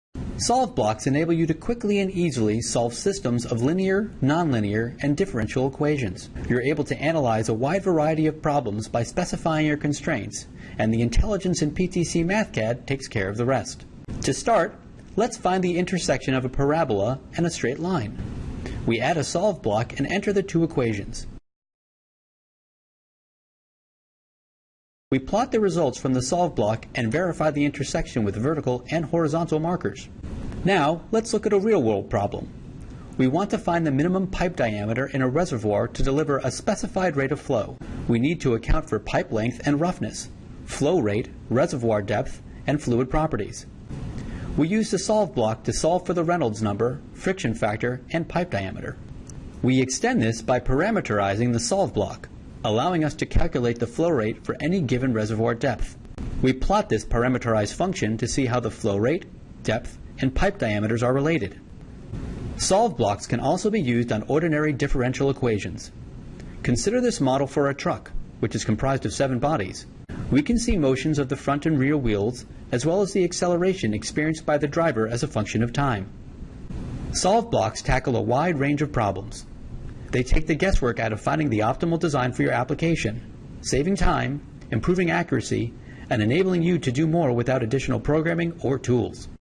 Solving Systems of Equations PTC Mathcad
Quickly solve systems of linear, non-linear, and differential equations.